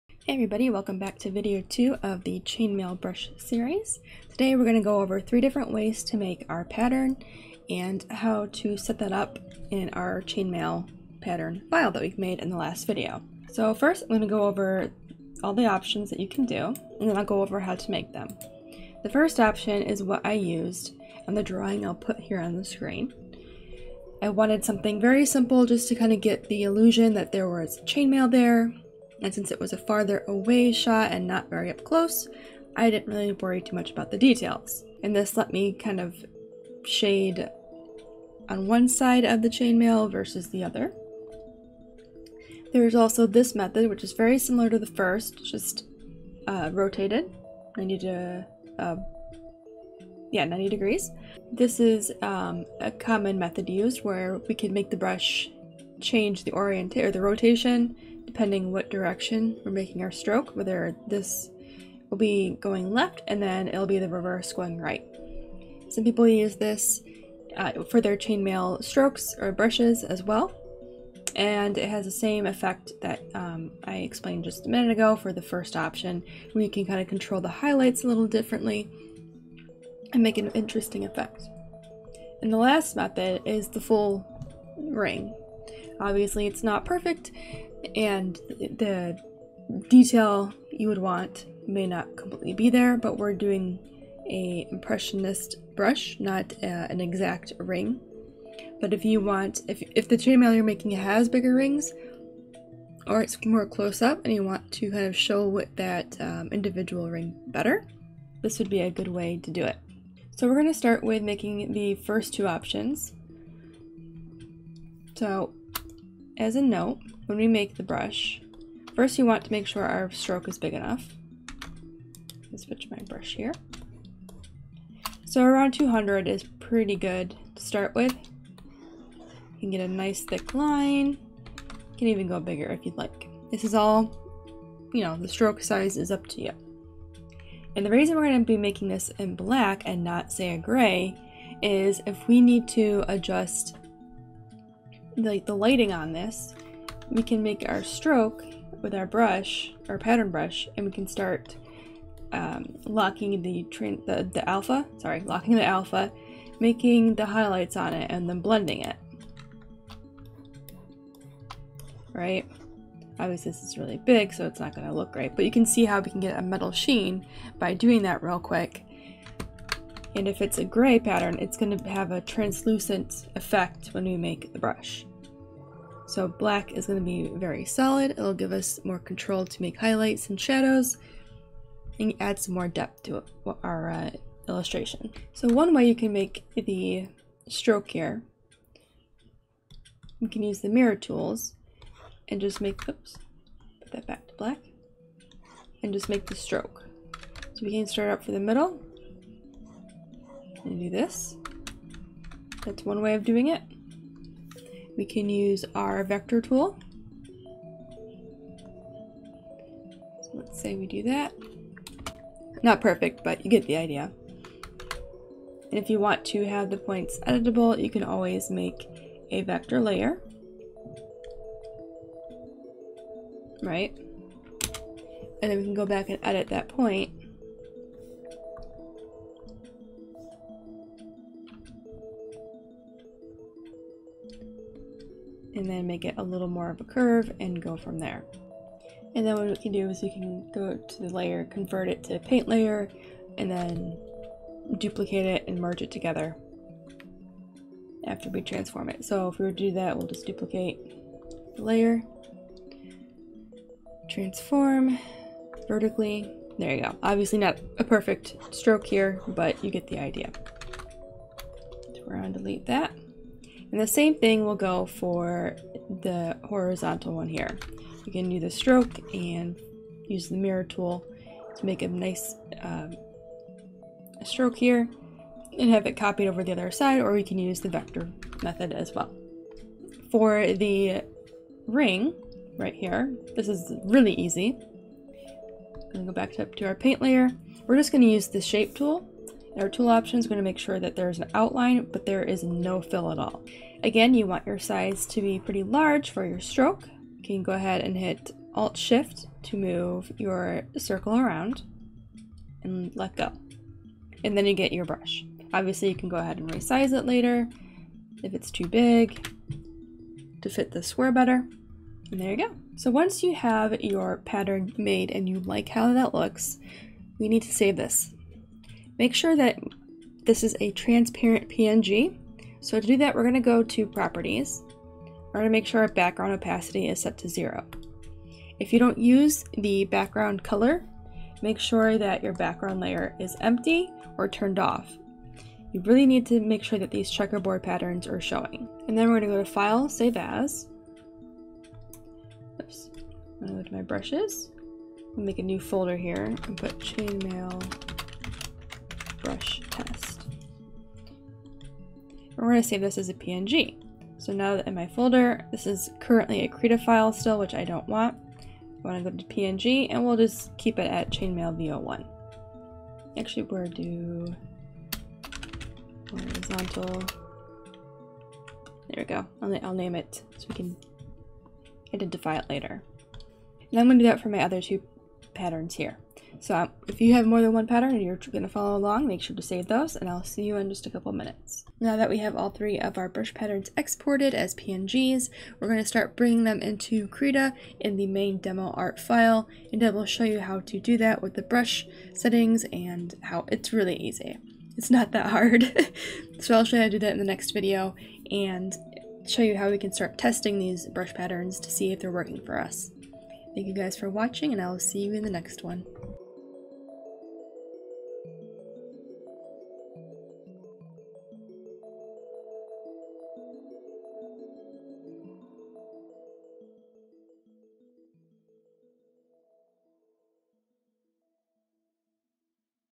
Krita Tutorial Series: How to Make a Chainmail Brush - Create Brush Patterns And Export Them
Video two of the chainmail brush series! We'll be going over three ways to make a pattern that can be used to create the illusion of chainmail in our illustrations in the software Krita, which is totally free! Make sure to download the program and check the first video for the file we'll be working with in this series.

00:00 - Intro
00:13 - Three Pattern Methods
01:50 - Brush Settings
03:05 - Using Mirror Tools for Curved Line Pattern
03:25 - Vector Method for Line Pattern
04:40 - Circle Chain
05:12 - Export Patterns
06:22 - Outro

Fifine USB Mic
Huion Tablet
SADES Headset